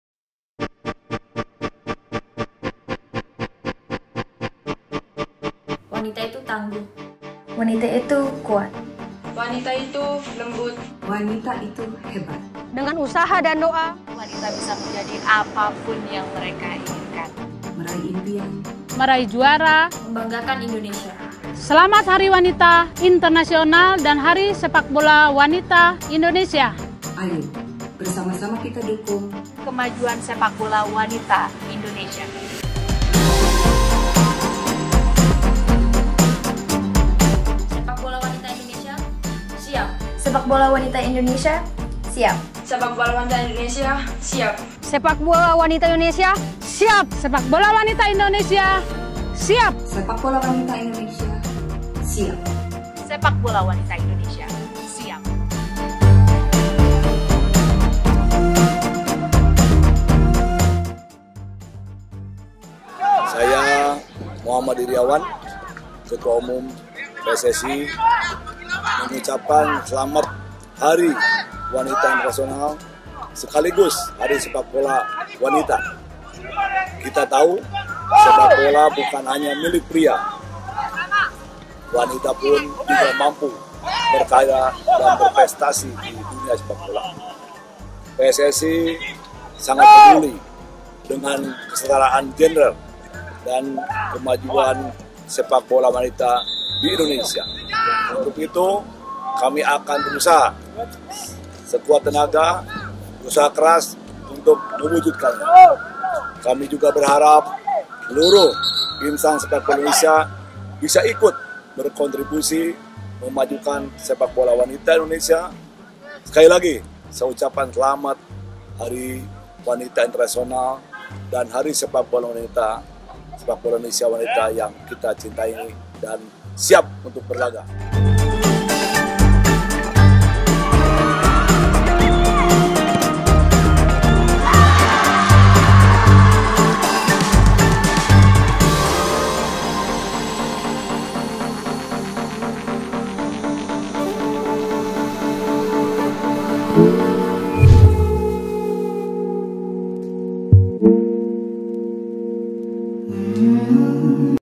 Selamat Hari Wanita Internasional dan Hari Sepak bola Wanita. ✨
Mari bersama-sama kita majukan sepak bola wanita Indonesia. Sepak bola wanita, siap! ❤️🇮🇩